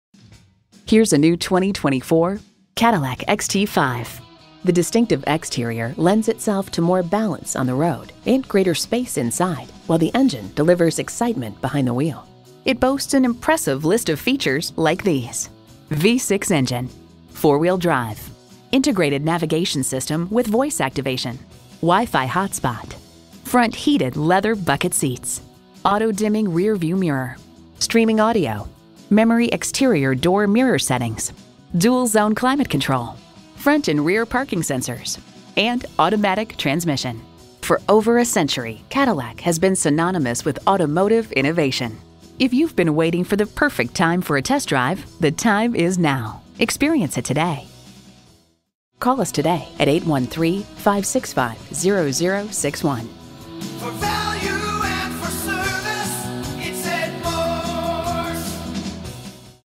New 2024 Cadillac XT5 Brandon FL Lakeland, FL
Year: 2024
Make: Cadillac
Model: XT5
Trim: AWD Sport
Engine: 3.6L V-6 gasoline direct injection, DOHC, variable valve control, regular unleaded, engine with cylinder deactivation and 310HP
Transmission: 9-Speed Automatic
Color: Stellar Black Metallic
Interior: Hmr Jet Black
Stock #: RZ727854
VIN: 1GYKNHRS6RZ727854

Address:
11020 Causeway Boulevard